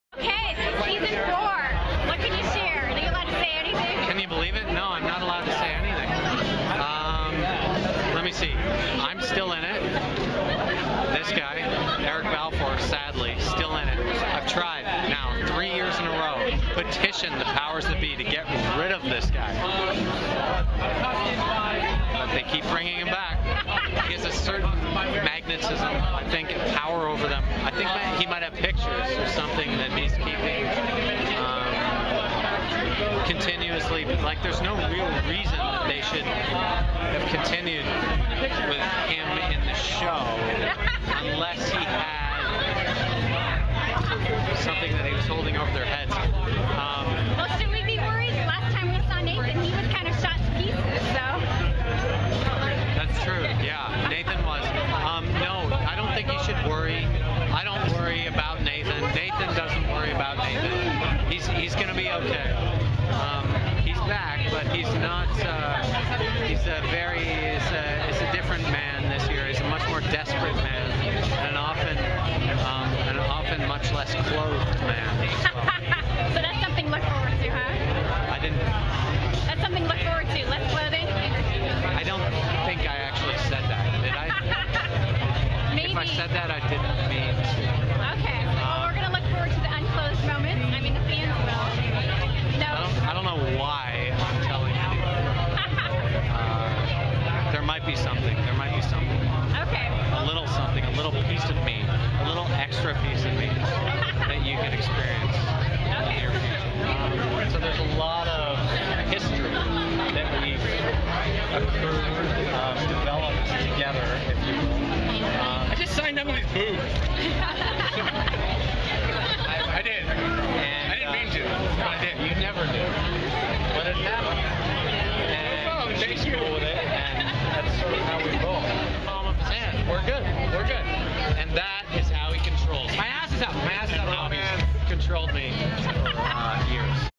"Haven" - Lucas Bryant
From SDCC 2013, interview with Lucas Bryant from the "Haven" party red carpet at San Diego Comic-Con re S4 teasers, with a few funny moments with his co-star Eric Balfour (look for the hug AND the kiss!)